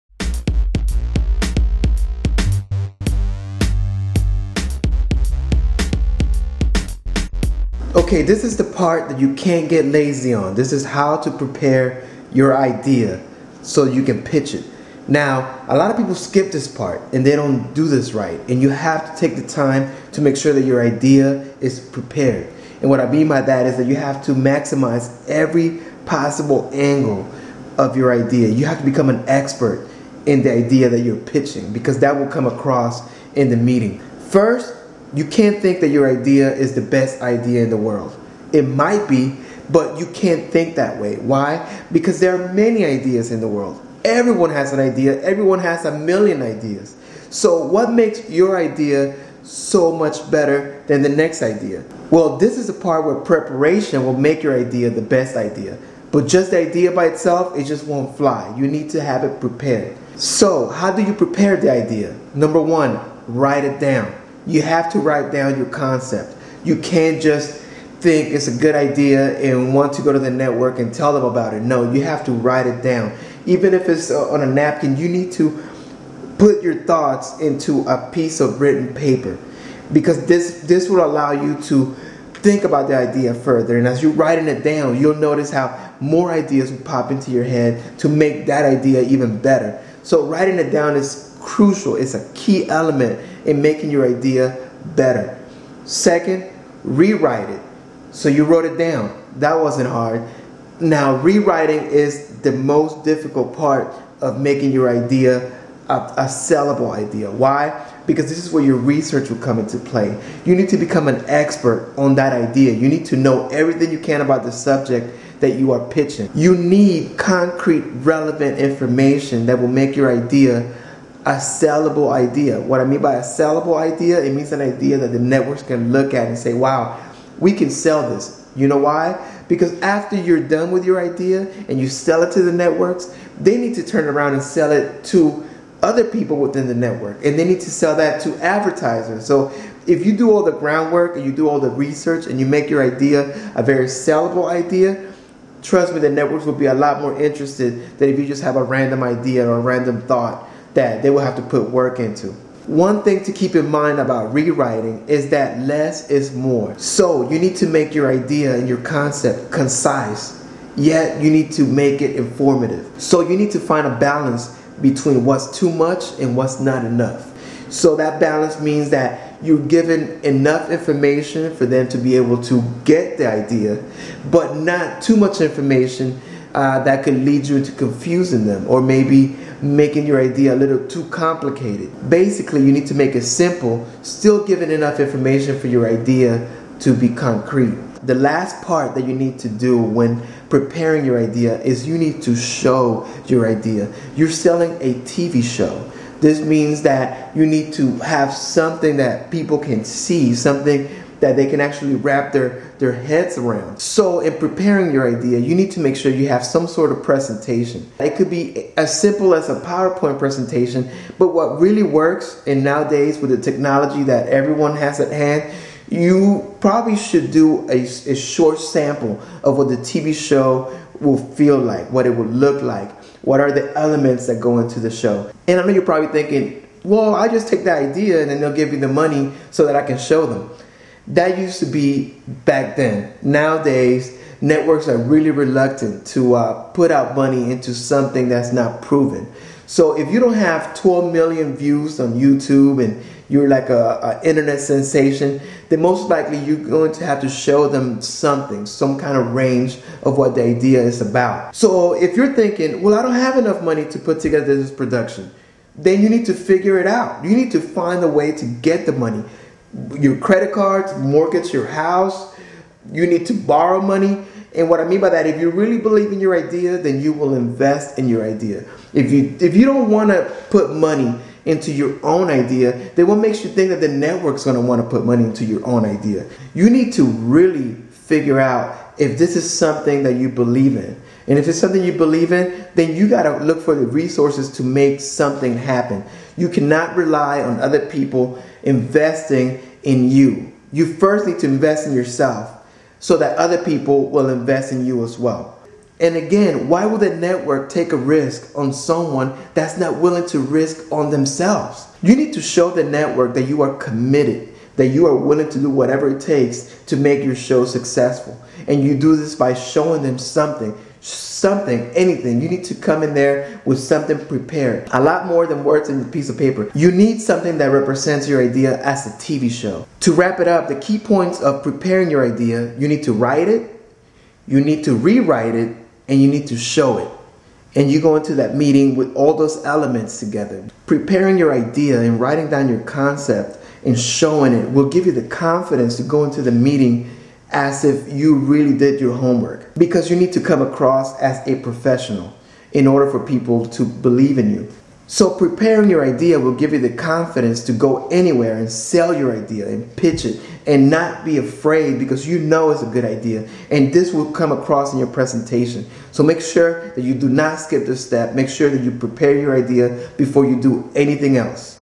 HOW TO PITCH A TV SHOW - Part 2 - Preparing Your Pitch
Preparing you TV show idea means that you will write, re-write your concept and have something to show the networks. You have to show them what your idea is, in one way or another.

The fact is that ideas are everywhere and everyone has them, so that doesn't make an idea that important. Actually there are so many ideas in the world that they're pretty much close to worthless. This holds true for TV show ideas. It's not very difficult to come up with a good TV show idea, and pretty much anyone can come up with one with a little thought. So how do you make your idea BETTER than the other millions of ideas out there? How do you present this idea in a way that seems like it's the GREATEST THING EVER?

It starts out with preparation. You just can't think of an idea and then call up the TV networks and tell them about it. I mean, you could, but probably won't get your very far - even if it truly is a great idea.  Networks don't buy ideas, they instead buy "the thinker behind the idea". This is important to understand because if you don't prepare your idea in a way that showcases YOU in the best light possible, then it makes your idea untrustworthy.

So how do you prepare your idea?

1) Put it in Writing - This means thoroughly writing down you concept down to every miniscule detail.  You'll notice as you are writing, that more elements to your idea start to surface. Write these down too.  This is an extremely important step in preparing your idea. Your writing will demonstrate that you have thought about all possible angles of your idea.

2) Re-Write It - Don't skip this step, since this might very well be what makes your idea something worthwhile. Read your idea, let some people you trust hear it or read it and ask for feedback. Read or watch similar ideas and dissect what makes them work. Do all the research you can about your concept. Now re-write your idea. If you did all your due diligence, now you should have a new insight or something different to add or take away from your idea. KEEP IT TO THE POINT - Don't take your idea in all sorts of direction. Keep it focused and remember this rule: LESS IS MORE!

3) Show It - A lot of people think they can just take their idea to the networks and that they will immediately get millions of dollars to make the idea come to life. If you have this in mind, you might want to reconsider your pursuit of success in the TV industry in general. It most likely not work the way you plan. The fact is today selling a TV show is quite competitive. People are taking fully produced TV series to TV Networks. The advantage is that having something already done will minimize the network's risks.

This leaves you to having something to show. That means a video, a trailer, some production picture, something! Invest in making your own idea happen. If you believe in your idea being so great, then it shouldn't be difficult for you to do whatever it take to bring it to life. After all, if you wouldn't put your own money in your idea, what makes you think that a TV network will want to?

When you invest time and money into your TV show idea it shows the network that you are serious and committed. It also allows them to visualize your idea within their programming. Because remember that when they buy a TV show, they have to turn around and sell it to advertisers. If they think that advertisers won't like your idea, they probably won't buy it. That's why you have to "wow" them with your presentation. You have to go in there prepared with something to SHOW.  It's a TV "SHOW" and not a book proposal. You have to show them your idea is awesome and that advertisers would be all over it. The network executives become your audience, so they have to like what you present to them, before they can even start thinking about buying your pitch.

DO NOT use the "I don't have money to produce anything" excuse. That's the lamest excuse you can use, ever. If you think you have a million dollars waiting on the other side of a door, wouldn't you do whatever it took to get that door opened? Of course! Same with your TV show idea. Use your credit cards, mortgage your house, borrow money, plain and simply go above and beyond to make your idea happen - of course, only if you think the idea is worth it. The television industry is a risk-taker's dwelling. If you don't have the guts to take risks, you should probably get a nine to five job somewhere and be content.

Of course you could put together a Powerpoint presentation and show them that. That might work - BUT if you really want to maximize your chances, take more than that into the meeting. A well put one-minute video trailer of your idea in motion is a lot more powerful than a 100-page Powerpoint presentation.